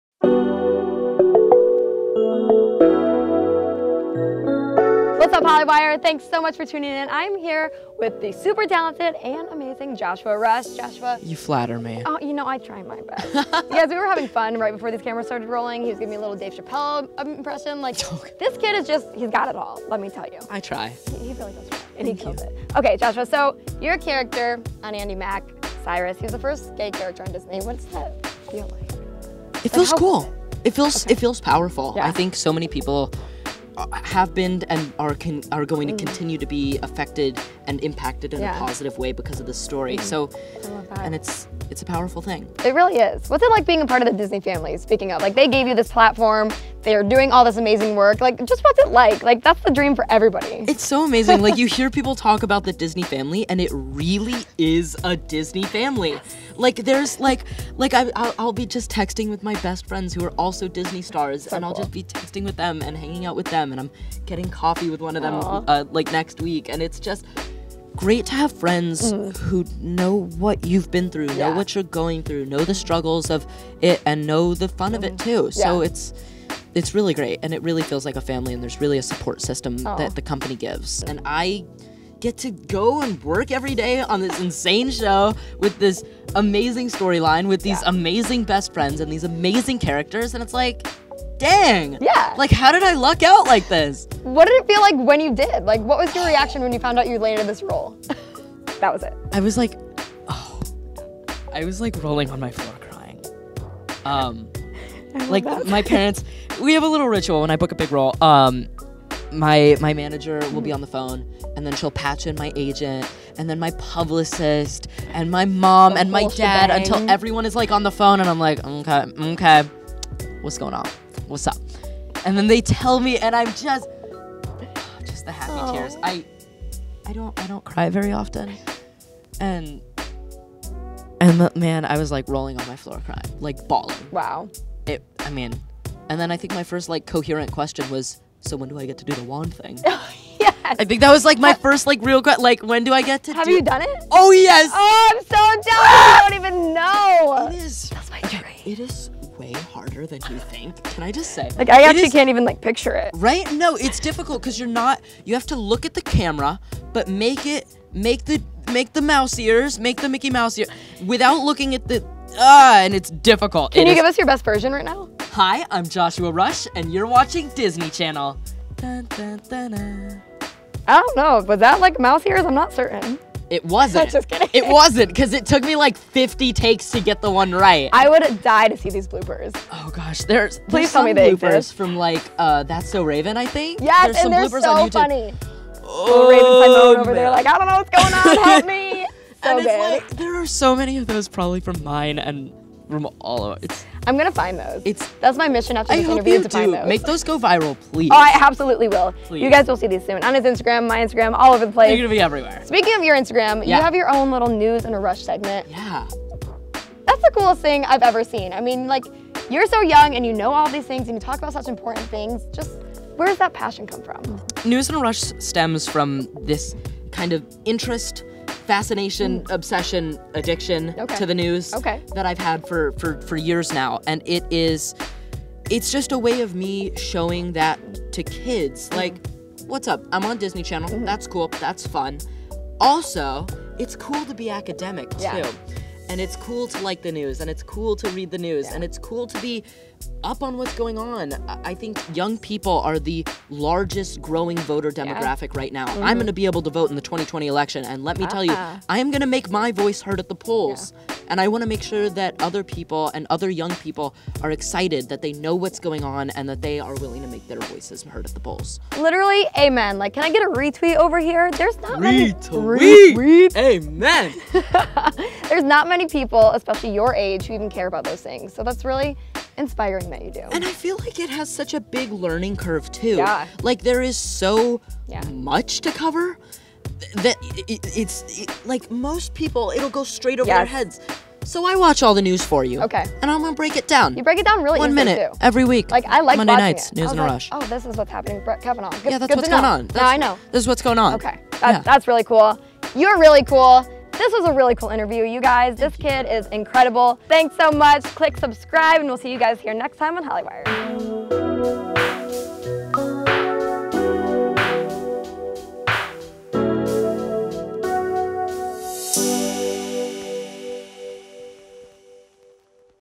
Joshua Rush Tells Us About Being The First Gay Character On A Disney Show! | Hollywire
We chat with Joshua Rush on the set of Andi Mack! His character is the first gay character on a Disney show. He loves being a part of the Disney family. He loves that other people understand what he's going through. Joshua Rush and his parents have a ritual when he lands a big role, they get on the phone with hi manager and surprise him with the good news. He has gotten to make the Disney mouse ears on tv. He also has a news segment, because he loves the news and wants to share his passion for the news with his fanbase.

For any goodies, send to:
Hollywire
9701 Wilshire Blvd # 8,

[Video Title]
[Video URL]